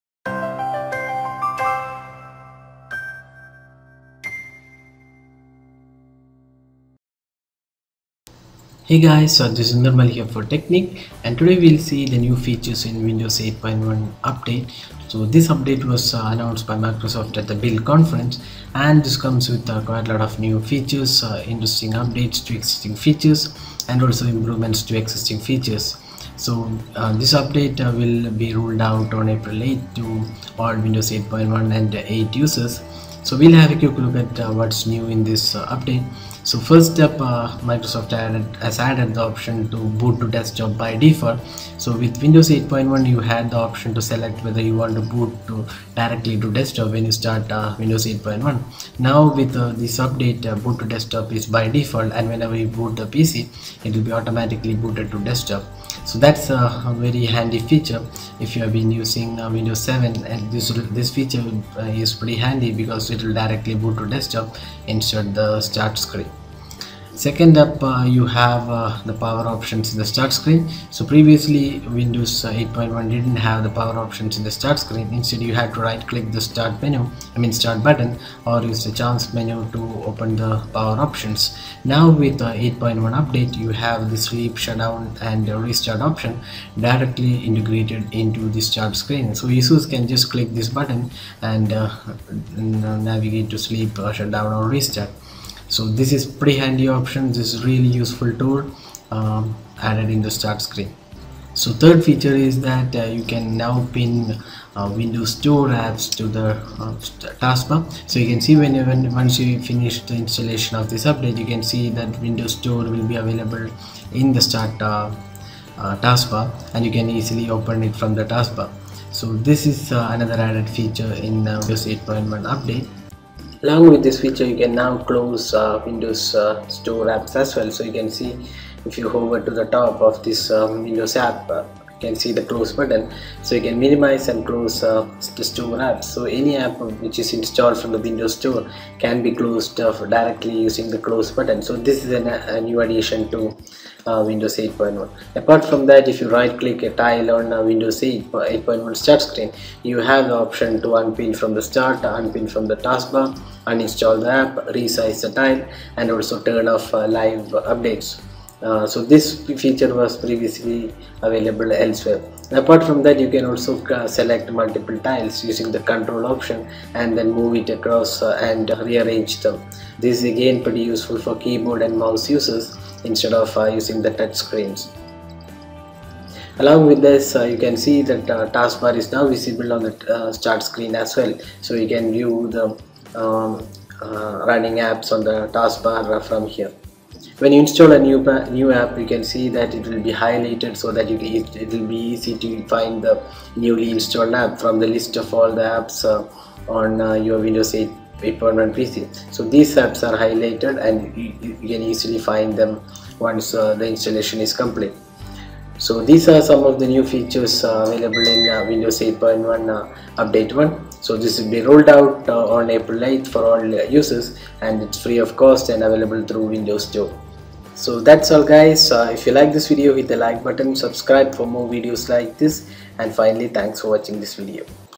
Windows 8.1 Update- New Features
A Quick look at the new features available in Windows 8.1 Update which was announced by Microsoft at BUILD and will be released to all users on April 8th as free update.